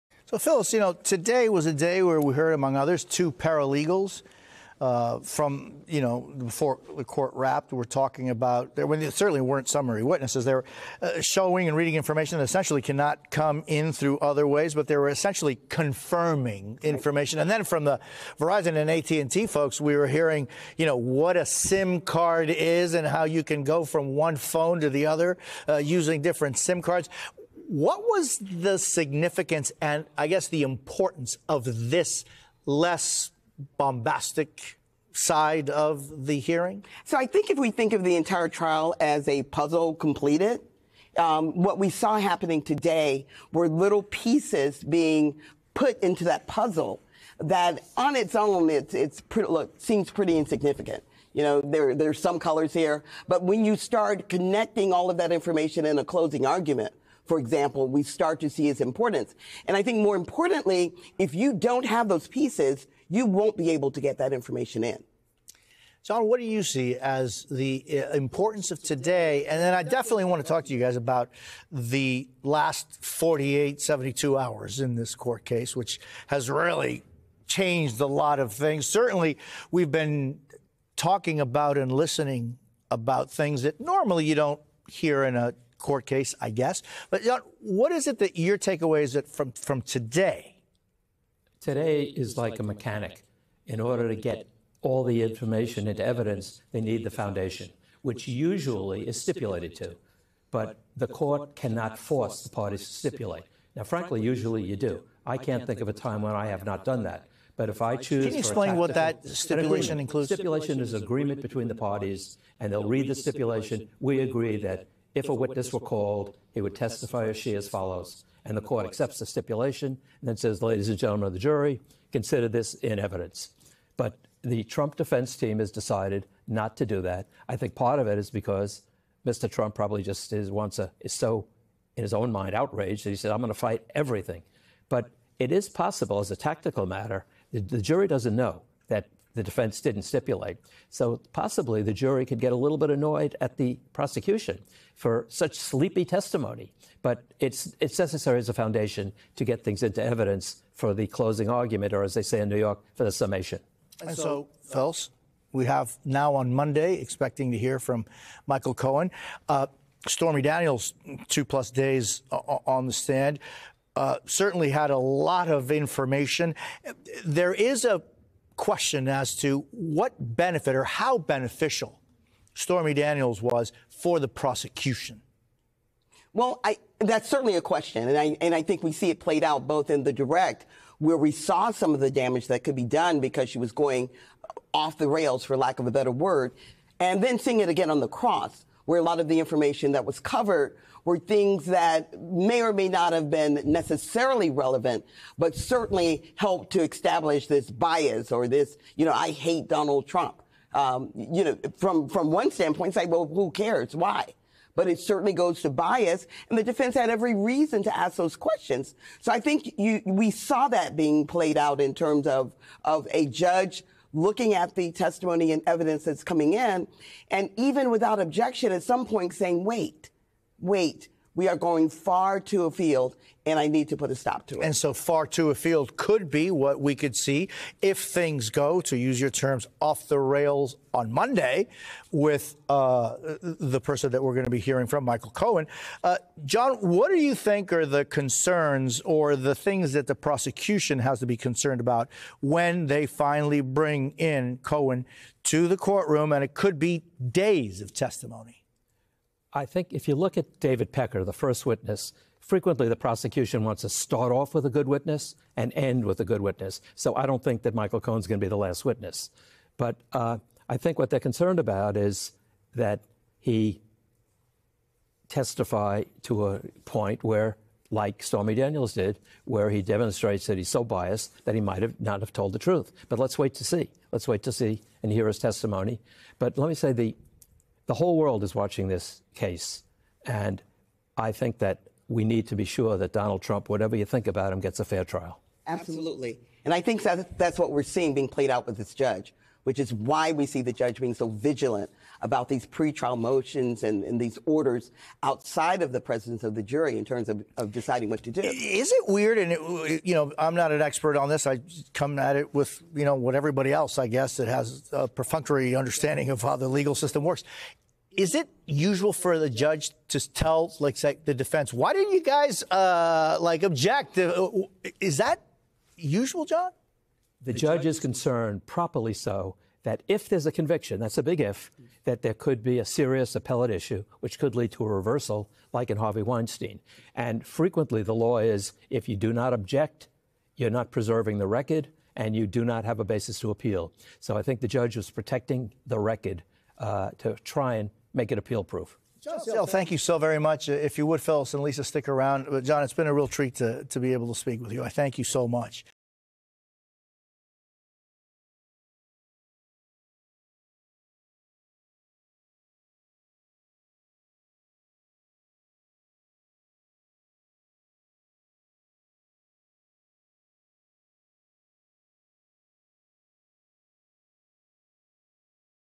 'The whole world is watching': Michael Cohen to testify Monday at Trump's trial
Southern District of NY former federal prosecutor Jon Sale and Florida International University clinical professor of law Phyllis Kotey share their takeaways from today's testimony in former President Trump's hush money trial and what to expect from Michael Cohen.